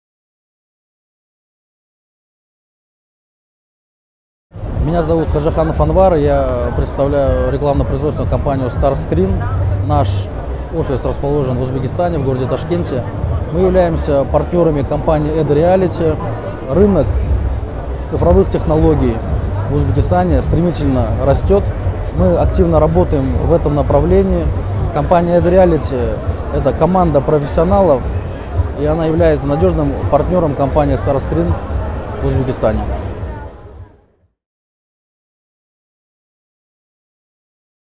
Директор Starscreen Анвар Хаджаханов о сотрудничестве с Addreality
Директор компании Starscreen Анвар Хаджаханов о сотрудничестве с Addreality